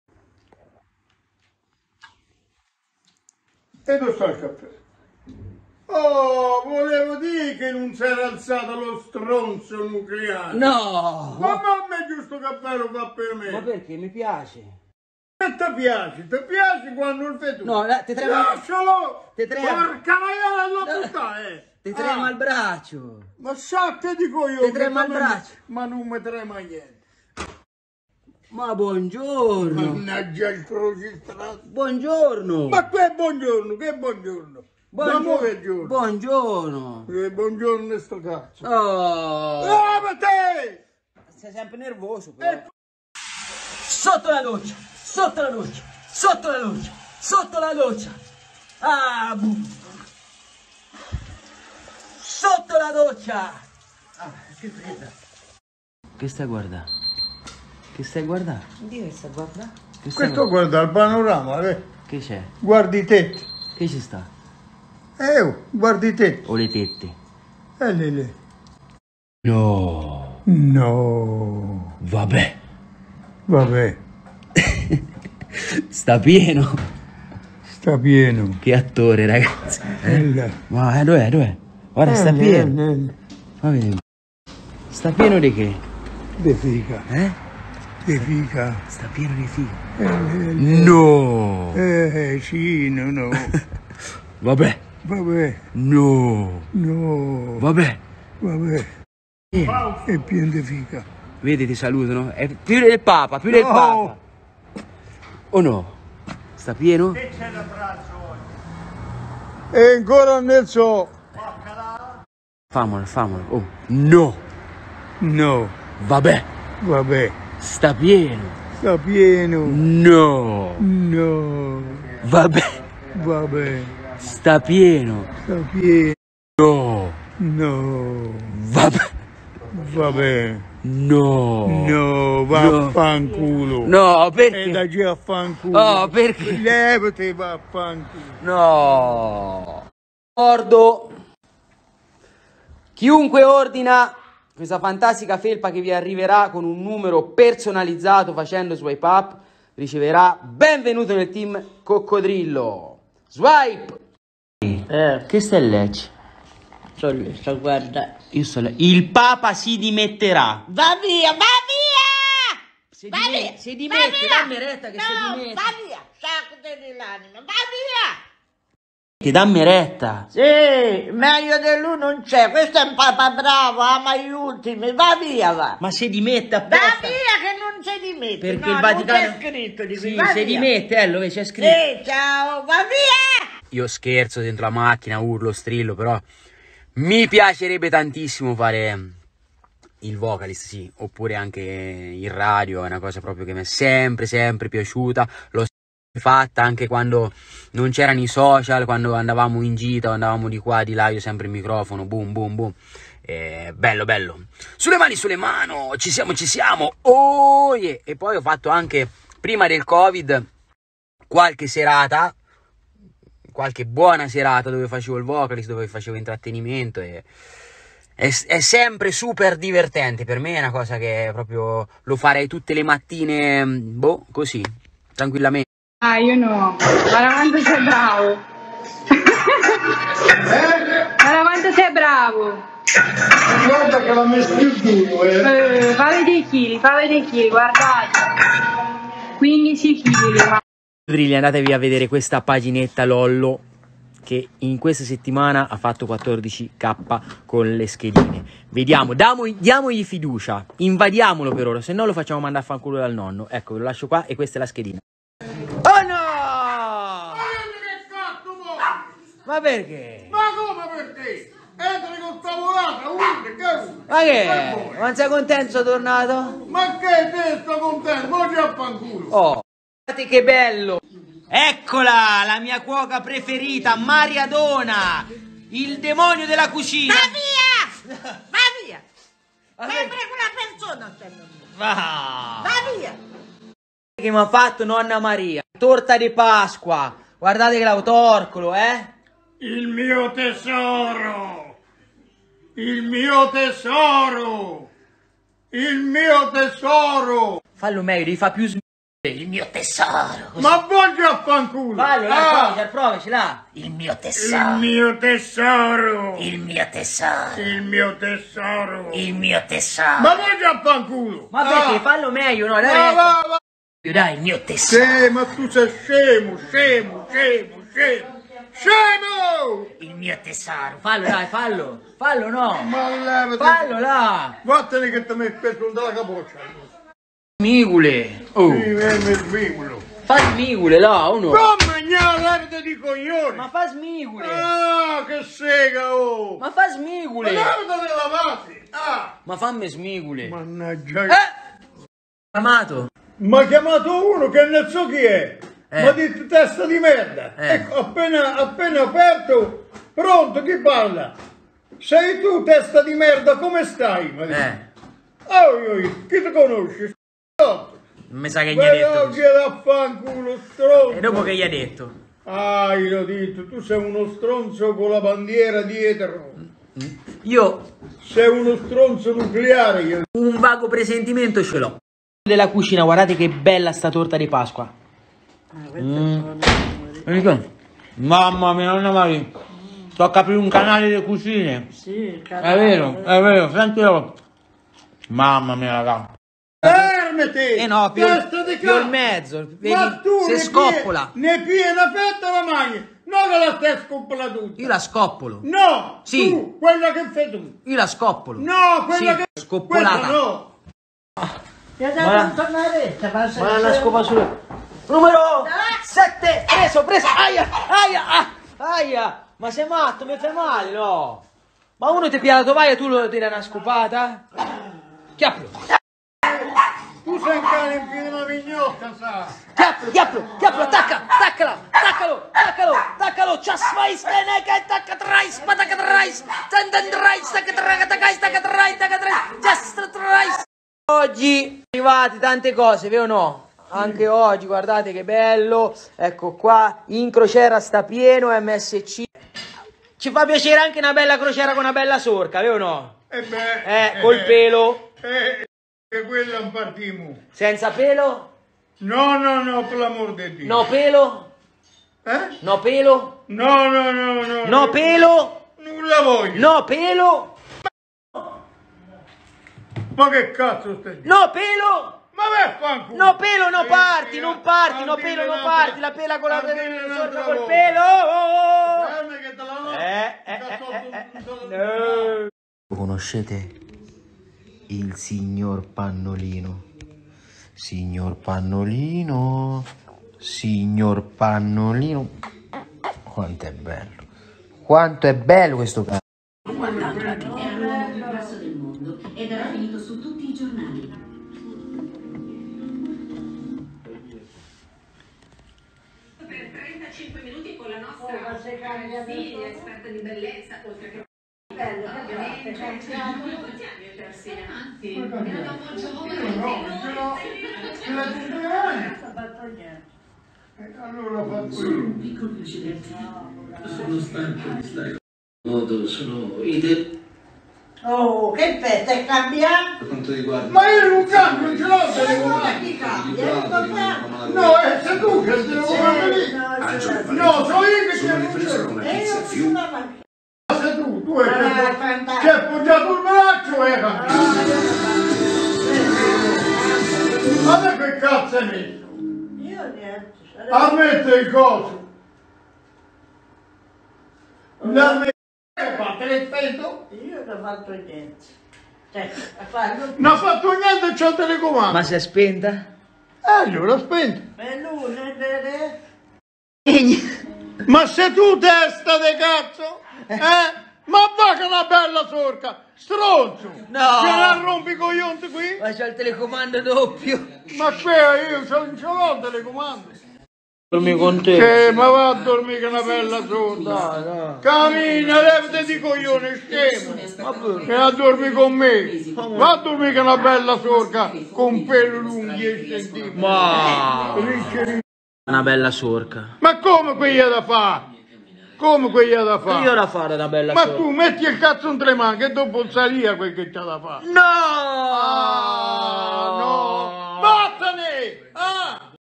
Un passato da vocalist - Pausa pranzo con Carletto: Ep. 64
Cosa vedrete in questa pausa pranzo?

Un caffè mattutino insieme, sotto la doccia, Nonno Faustino guarda il panorama, team Coccodrillo, il Papa si dimetterà?, Nonno in palestra, Mariadona il demonio della cucina, uno S... nucleare, la torta di Pasqua, scoppolata, una bella crociera..., al buio a fare scherzi..